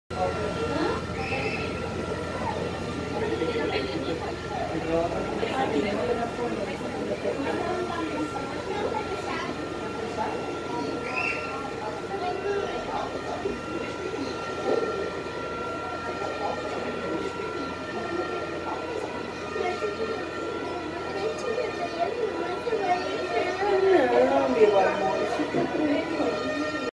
Bombeiros realizam o combate ao incêndio em apartamento de São José
Imagens: Patrícia Gomes/Reprodução